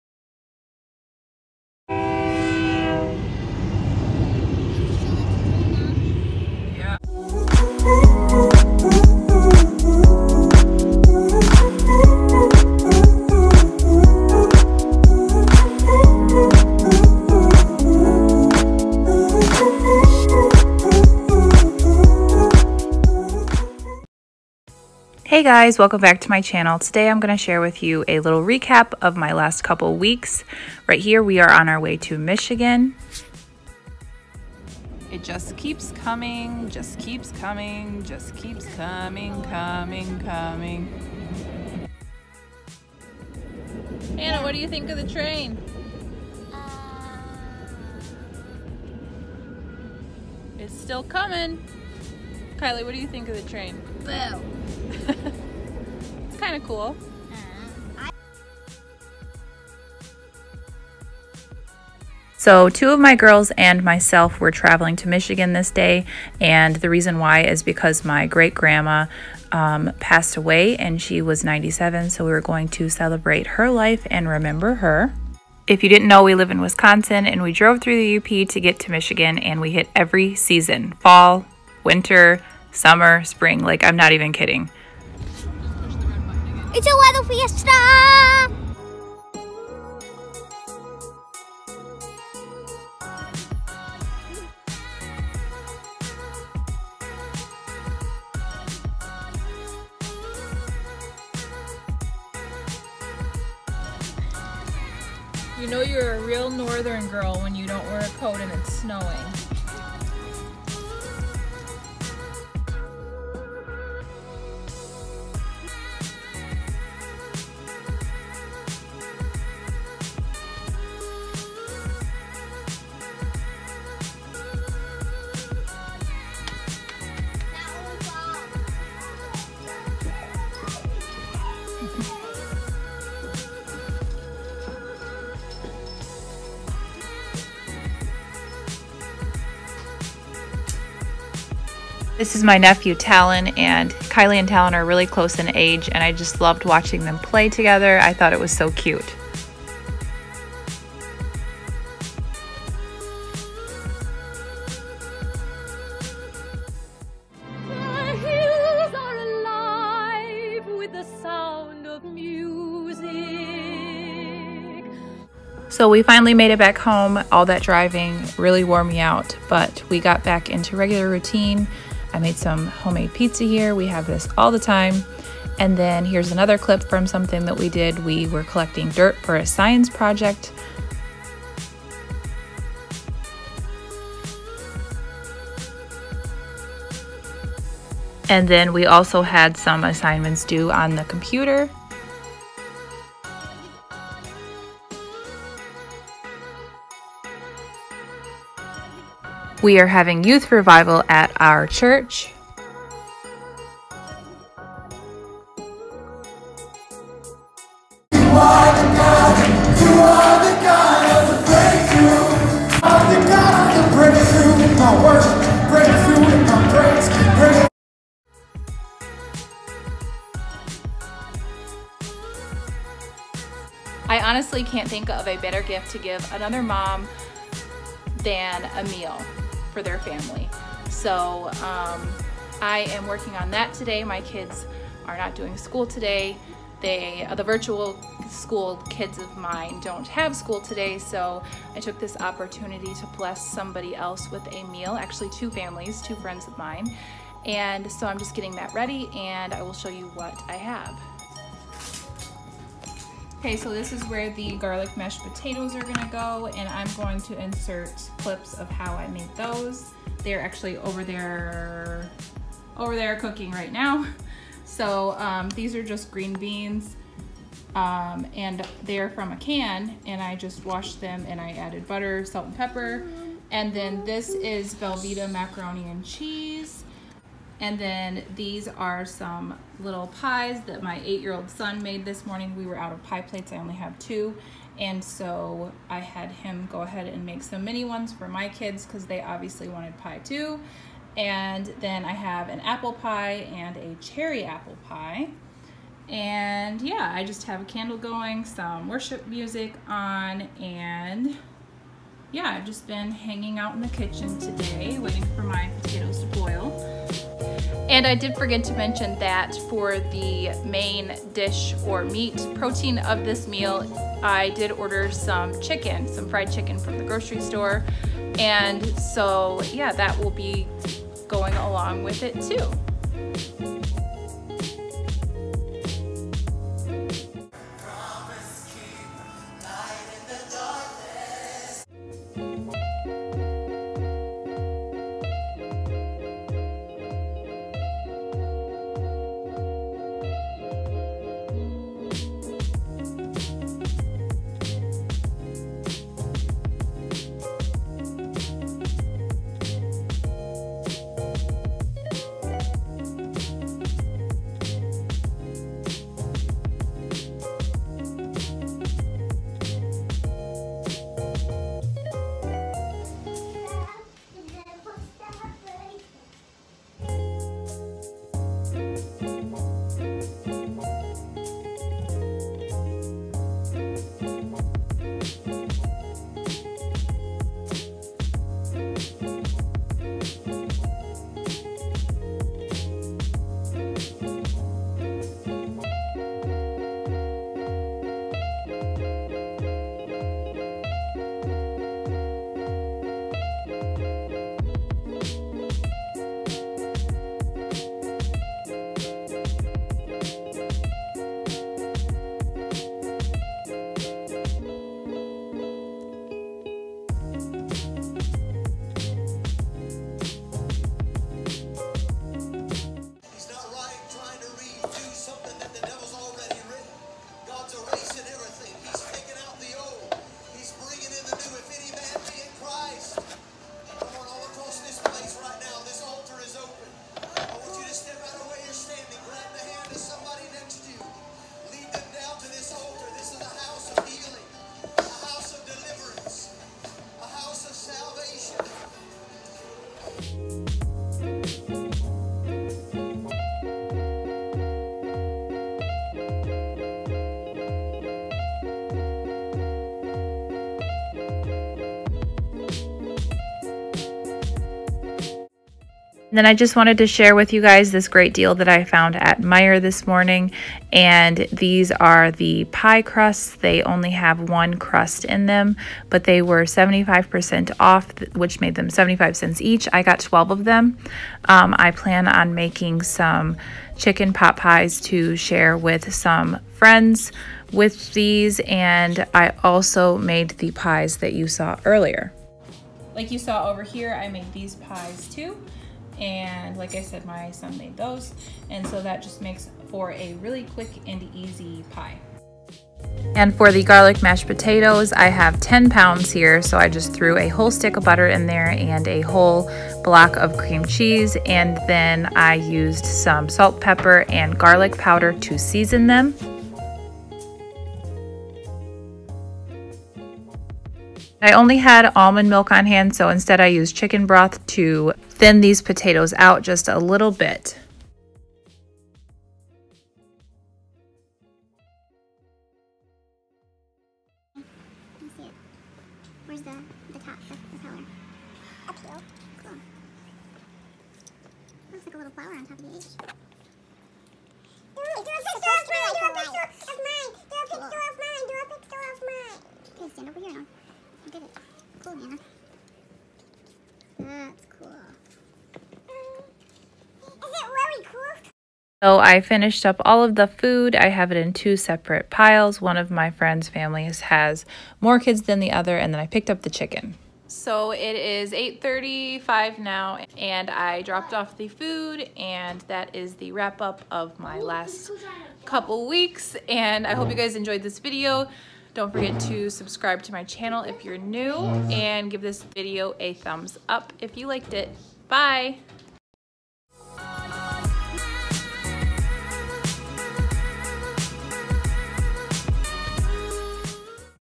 A RECAP OF MY LAST COUPLE WEEKS // DITL OF A STAY AT HOME MOM OF FIVE
HEY FRIENDS!!
Thanks for stopping by, I have been a little bit MIA the last couple weeks, so I complied a bunch of clips that I took. Life happened, and I'm still here. I have TONS of videos planned for the next couple months, so stick around!